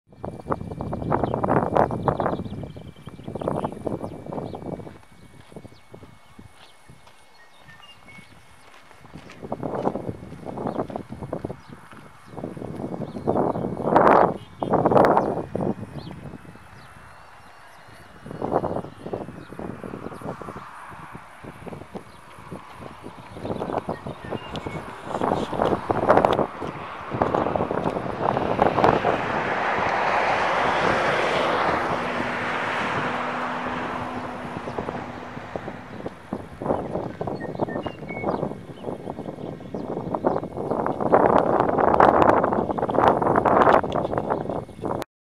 New Zealand ハウェア湖
ニュージーランドのハウエア湖です。
鳥のさえずりが聞こえ、美しい湖でした。